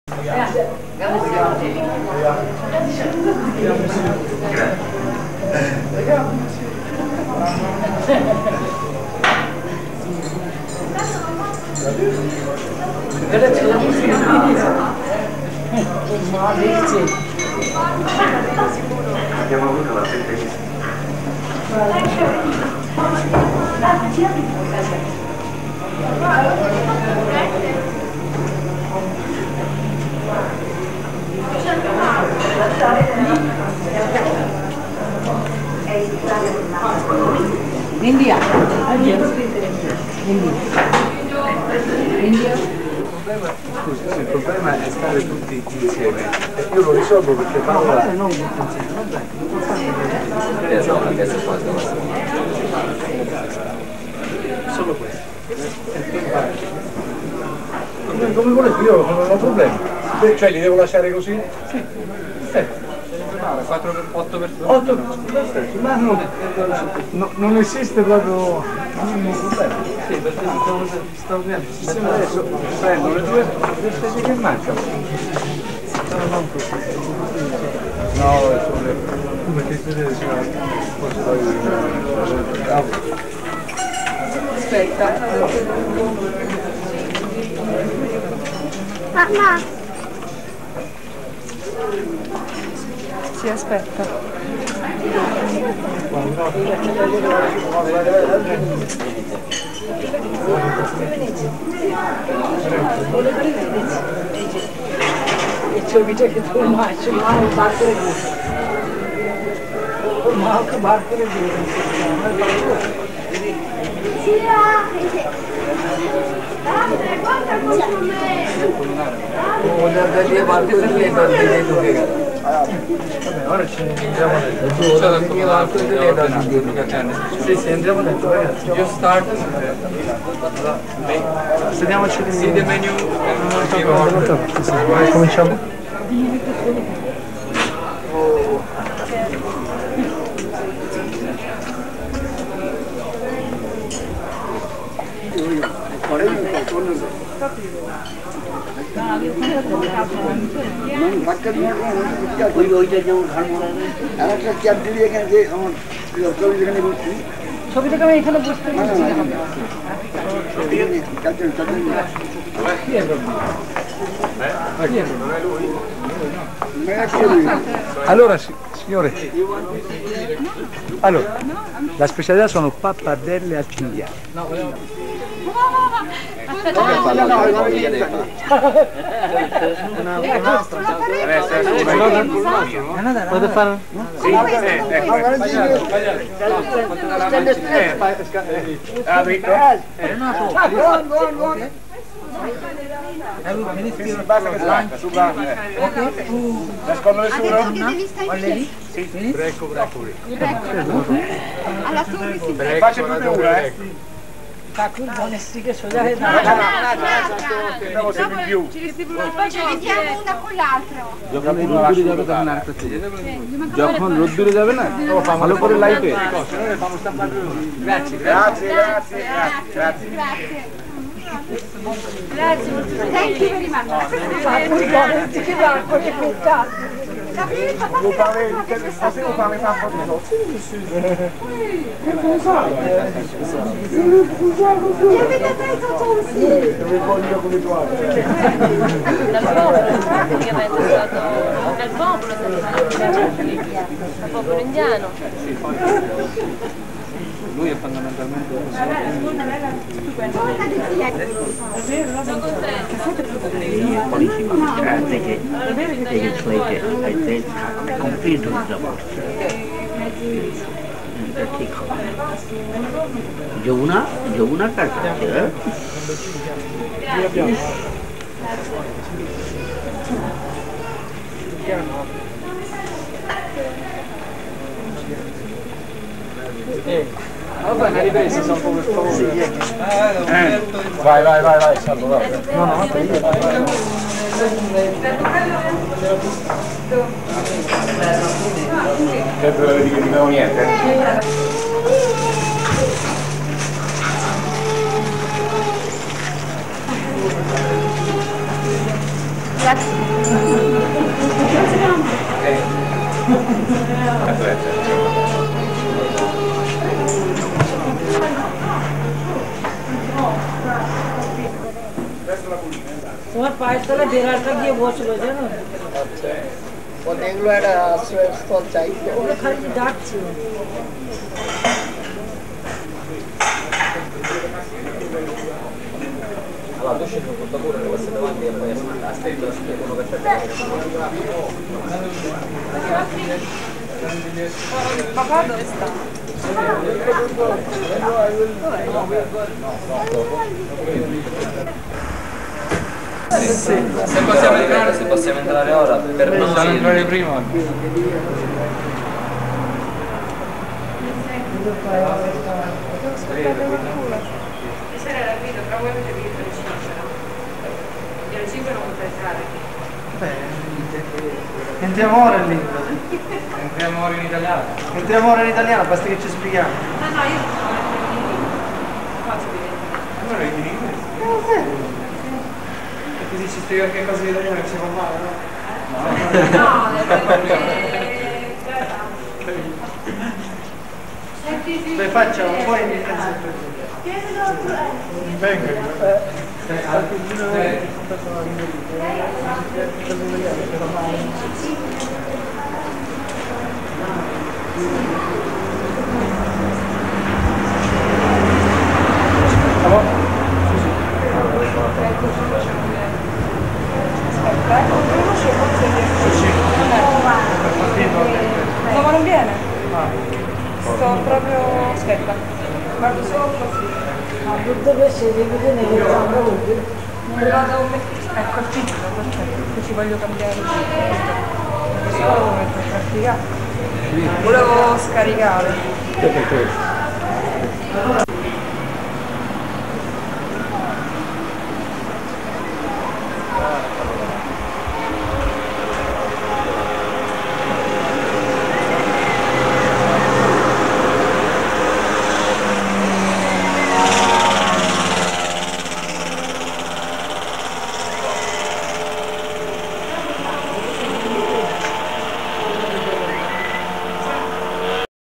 YOGI GURU BHAGABAN KARUNAVATAR SRI SRI THAKUR BRAHMACHARI ANILANANDAJI MAHARAJ IN ITALI. PART-I
YOGI GURU BHAGABAN KARUNAVATAR SRI SRI THAKUR BRAHMACHARI ANILANANDAJI MAHARAJ ON THE OCCASION OF ESTABLISHING KRIYAYOGA ASHRAM IN ITALI.